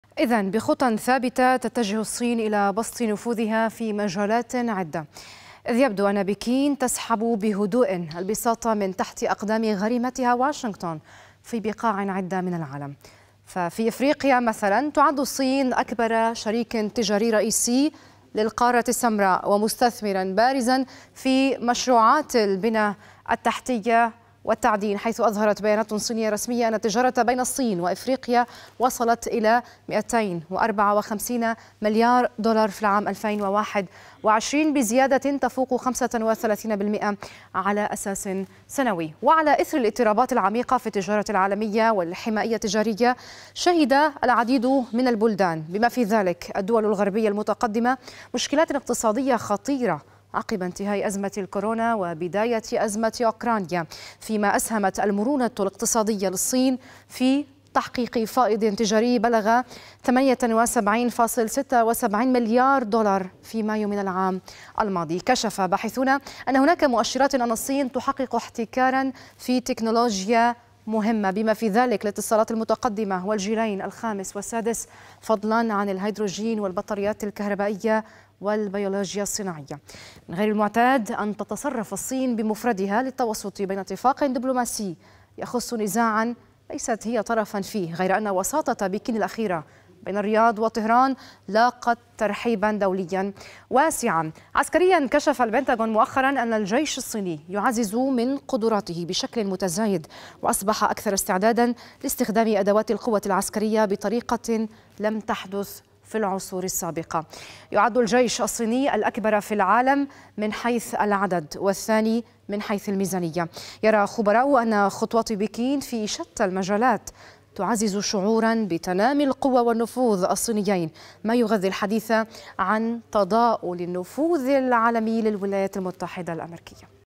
دور متزايد للصين.. بين السياسة والاقتصاد والتكنولوجيا
بعد الرعاية الصينية للاتفاق بين السعودية وإيران، يبرز الحديث عن الدور المتزايد للصين في المنطقة والعالم..
دورٌ يبرز في مجالات عدّة منها السياسية والاقتصادية والتجارية وكذلك التكنولوجية.
ما أبرز الأدوار التي تقوم بها بكين عالمياً في هذه المجالات؟